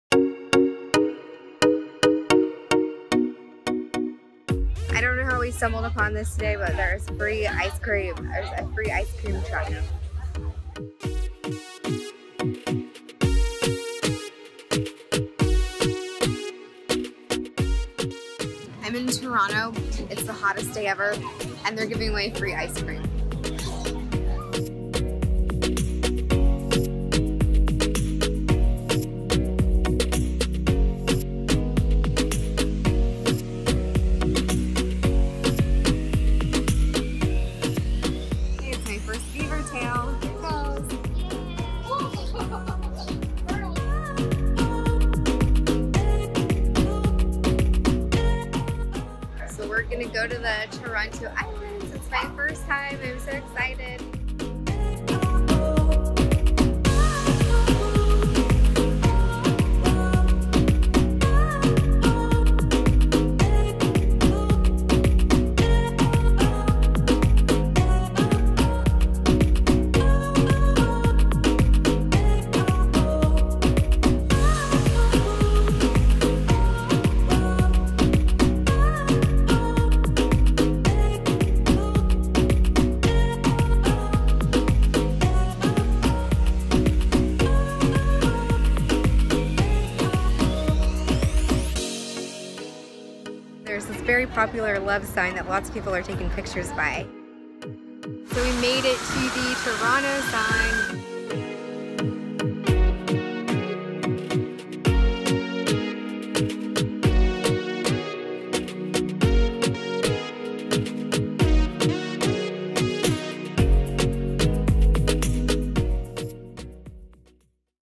Exploring Toronto: Fun Things to Do on a Summer Day - Snaps From My Phone
Here are some snapshots of my first time exploring Toronto! This time my cousin was kind enough to show me around. What a beautiful city, can't wait to go back and explore more of Canada.

Please check out my blog post for full details (including travel time) for planning a layover in Tokyo

Here's a bit of what you'll find on this video. Enjoy!

1. Harbourfront Centre - free ice cream made my day!
2. Toronto Islands - took a short ferry ride to explore these
3. Beaver Tails - my first time trying these doughy treats
4. CN Tower - snapped a few shots of this iconic tower from the islands & downtown
5. Nathan Phillips Square - love that Toronto sign -- especially when they light it up!
6. The Distillery District - cool art murals & sculptures
7. Urbinz Toronto  -- funny Canadian socks
8. Fugo Desserts -- s'mores ice cream cone!

Happy Travels!

Aliya